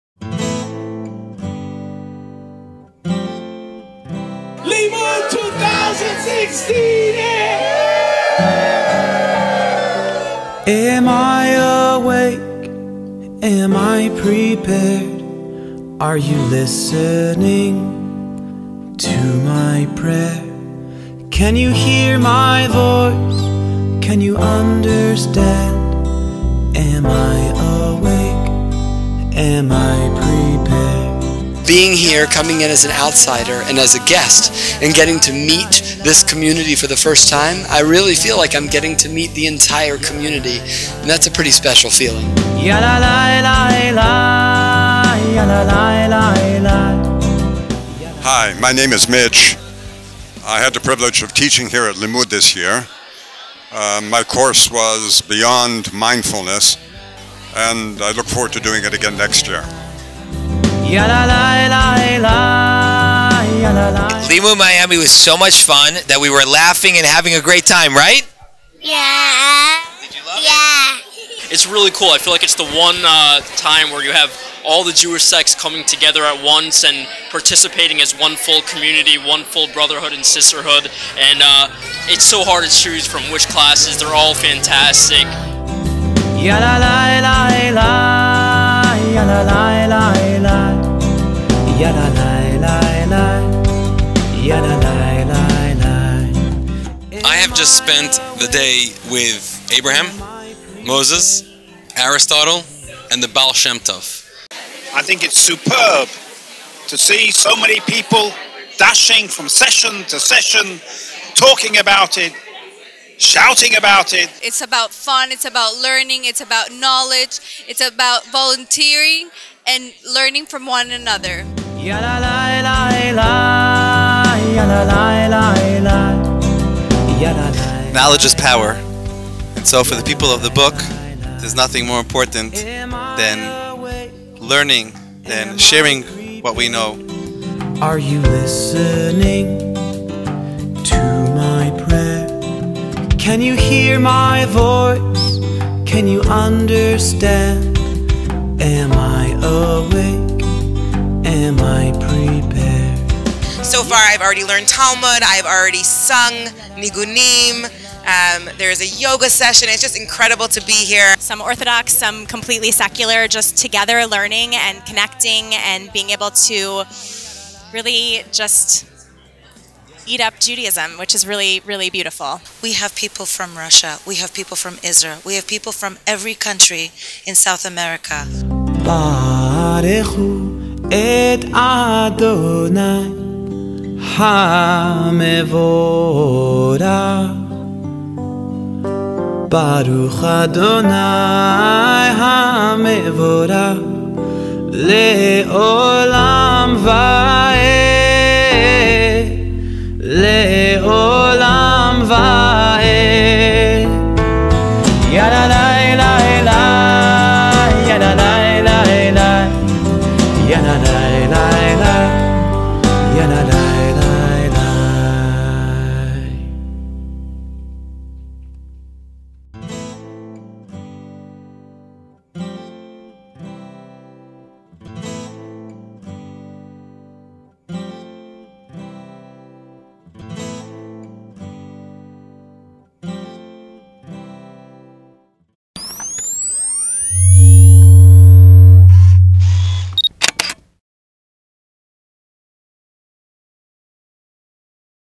Corporate and Conference Event Video Coverage for Limmud Miami 2016
Production Company I FancyDesign.com
Cinematographer/Producer I Yaniv Feldman
Client I Photos by Carlos Chattah
Location I FIU Kovens Conference Center in North Miami.

The recent Limmud Miami 2016 drew more than 850 people at the Kovens Conference Center on Florida International University's Biscayne Bay Campus and received a positive reception.
This learning festival, which made its local debut two years ago, has been implemented in communities throughout the world. It featured a set of workshops and presentations with Jewish educators, performers and teachers from around the world. Its broad range of themes included arts & culture, spirituality, science, Torah, history, politics and educational programs that were tailored for all ages.

Captured, B-roll , our team also captured speakers, activities, and testimonials, aerial/drone footage was added to the video.